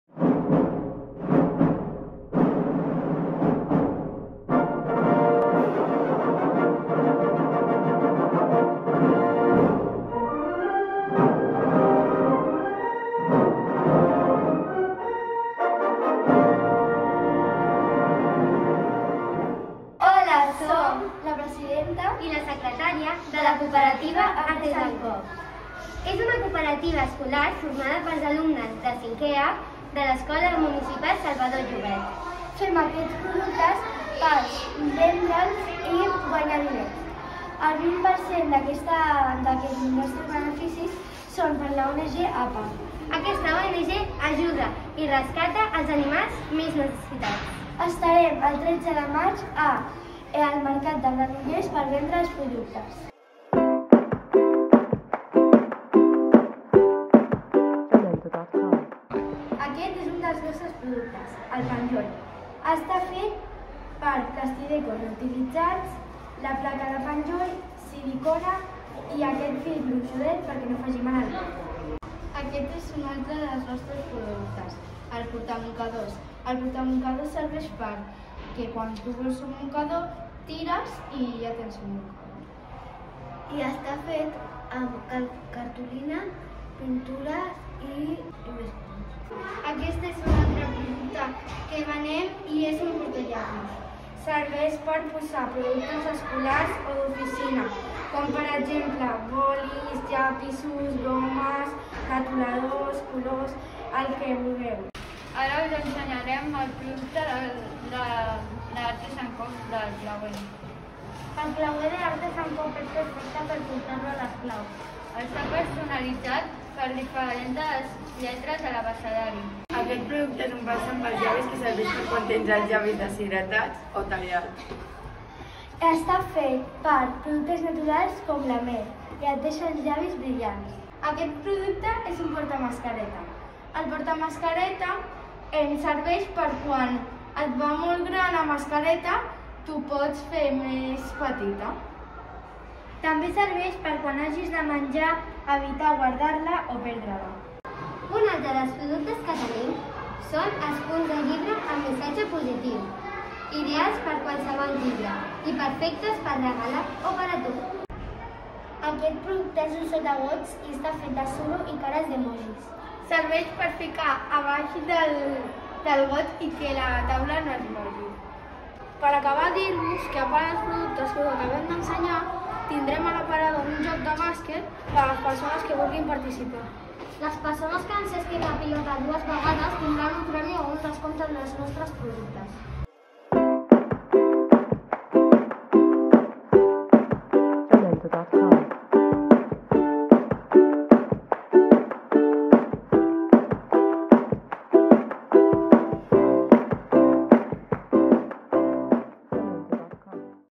Cooperatives escolars 2020/2021: Artesancoop 5è A (escola Salvador Llobet)
Cooperatives escolars 2020/2021
Granollers s’ha sumat un any curs més al projecte Cultura Emprenedora a l'Escola a través de la creació de cooperatives escolars organitzades i gestionades pels alumnes de 5è i 6è de primària. En total s’han creat 11 cooperatives escolars de 4 escoles de la ciutat que participen en aquesta iniciativa de la Diputació de Barcelona i que desenvolupen amb el suport dels serveis d’Educació i de Promoció econòmica de l’Ajuntament de Granollers.
El projecte Cultura Emprenedora a l'Escola, que arriba enguany a la 10 edició, té com a objectiu incrementar la creativitat, la innovació i l'esperit emprenedor. La iniciativa, impulsada per la Diputació de Barcelona, aposta pel model cooperatiu com a filosofia empresarial de base, perquè aporta un sistema de gestió democràtica i participativa i reforça una vessant de responsabilitat social. Per això, el procés de creació, organització i gestió d'una cooperativa a l'escola té la finalitat de fomentar l'esperit emprenedor que ajudi a adquirir valors, habilitats i capacitats com competència lingüística, cultural, social i ciutadana, matemàtica i competència en el coneixement i la interacció amb el món físic.